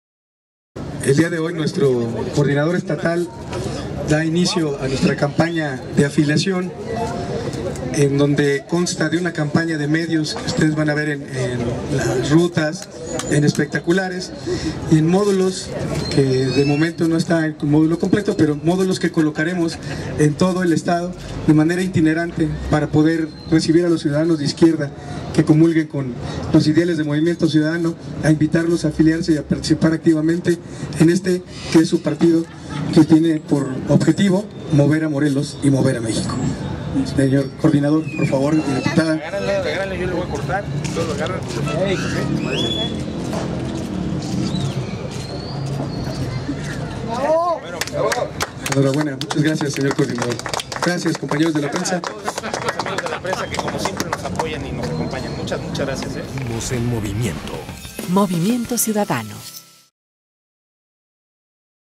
Inauguración campaña Afíliate - 15abr2013
Inauguración de la campaña Afíliate en la plaza de Armas (zócalo de Cuernavaca)
Con:
de izq. a der: Lic. Cuauhtémoc Nuñez (Síndico de Xochitepec), Lic. Eduardo Horacio López (Regidor de Temixco), Lic. Jessica Ortega (Integrante de la Comisión Operativa Estatal), Lic. Luis A. Machuca (Coordinador Estatal), Lic. Marco Valdin (Regidor de Cuernavaca) y la Lic. Amparo Loredo (Regidora en Jiutepec)
¡SEGUIMOS EN MOVIMIENTO!